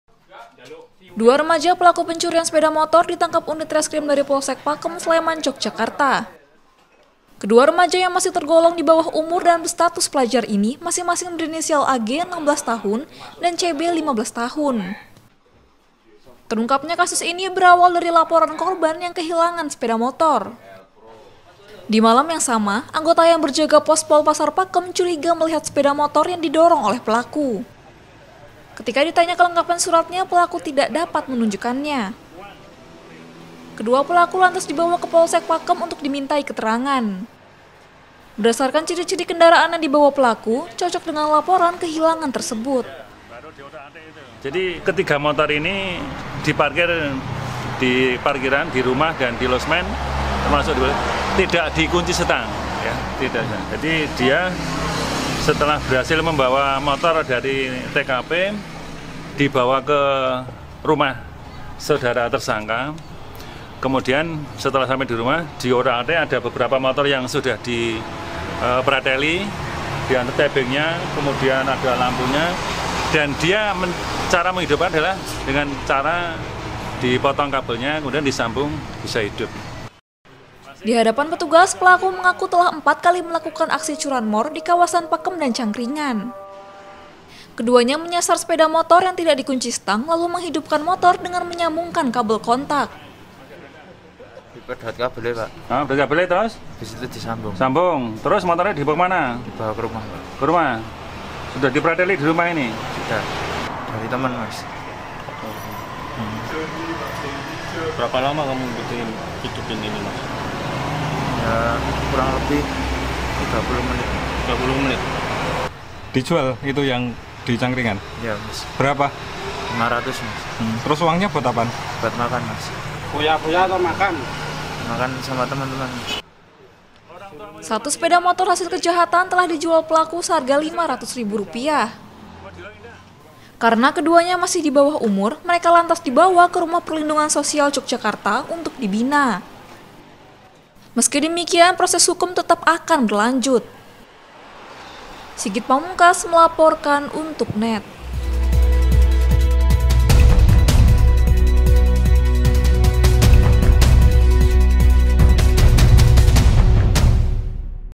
WASPADA!! Pelajar ‘Dibawah Umur’ Nyolong Motor - NET YOGYA
2 remaja pelaku pecurian sepeda motor ditangkap unit reskrim dari Polsek Pakem, Sleman, Yogyakarta. Kedua remaja yang masih tergolong dibawah umur dan berstatus pelajar ini masing-masing berinisial ‘AG’16 tahun dan ‘CB’ 15 tahun.

Terungkapnya kasus ini berawal dari laporan korban yang kehilangan sepeda motor. Dimalam yang sama anggota yang berjaga pospol pasar pakem curiga melihat sepeda motor yang didorong oleh pelaku. Ketika ditanya kelengkapannya suratnya pelaku tidak dapat menunjukkannya.

Kedua pelaku lantas dibawa ke Polsek Pakem untuk dimintai keterangan. Di hadapan petugas, pelaku mengaku telah 4 kali melakukan aksi curanmor di kawasan Pakem dan Cangkringan. Satu sepedamotor hasil kejahatan telah dijual pelaku seharga Rp 500 ribu. Karna keduanya masih dibawah umur, mereka lantas dibawa kerumah perlindungan sosial Yogyakarta untuk dibina. Meski demikian. proses hukum tetap berlanjut.

57 UHF Pkl. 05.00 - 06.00